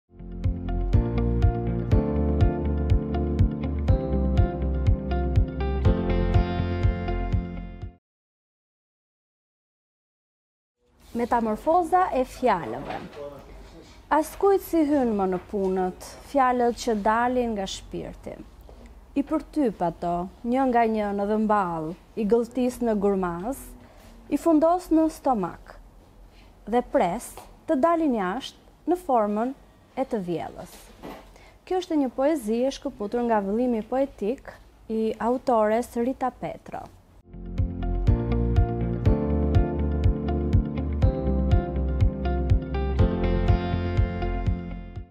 Maratona e leximit në librarinë Albas
Nanila lexon një pjesë nga libri i saj i preferuar...